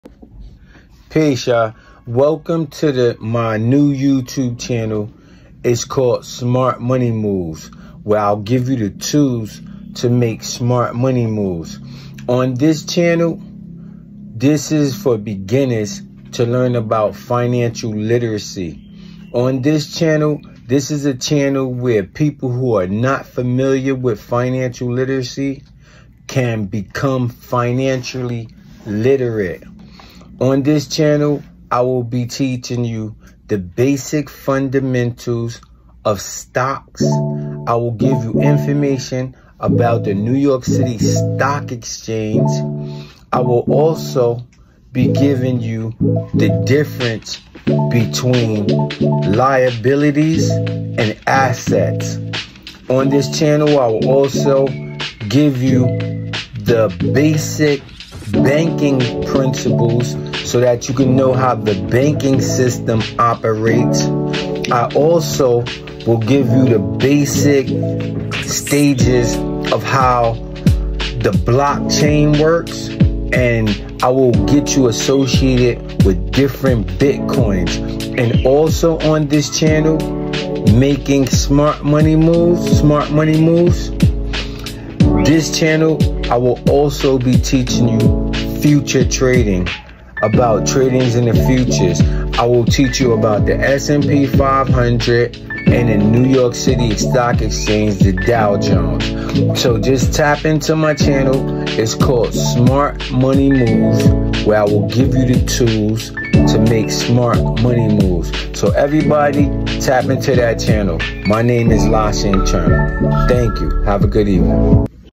SMART MONEY MOVES ( introduction to my new channel )
SMART MONEY MOVES
Where you will be given the tools to make,
SMART MONEY MOVES

On this channel you will be taught to become
FINANCIALLY LITERATE.
This channel is a channel we’re people who are not FINANCIALLY LITERATE, can become FINANCIALLY LITERATE, be utilizing the tools and principles given to you on this channel to
ELEVATE YOUR FINANCIAL STATUS AND CREATE GENERATION WEALTH.
On this channel you will be taught.
1. THE DIFFERENCE BETWEEN ASSETS AND LIABILITIES.
2.  THE BASIC FUNDAMENTAL OF STOCKS AND YOU WILL ALSO AQUIRE THE BASIC FUNDAMENTALS OF HOW THE NEW YORK CITY STOCK EXCHANGE OPERATES.
3  On the channel we will go through different
BANKING PRINCIPLES & HOW THE BANKING SYSTEM OPERATES.
4 . On the channel you will also acquire information BITCOINS & HOW BITCOINS WORK.
5. On this channel you will also be learning
THE BASIC PRINCIPLES ON REALESTATE.
6 . ONn this channel you will be also given information on FUNDAMENTALS OF FUTURE TRADING.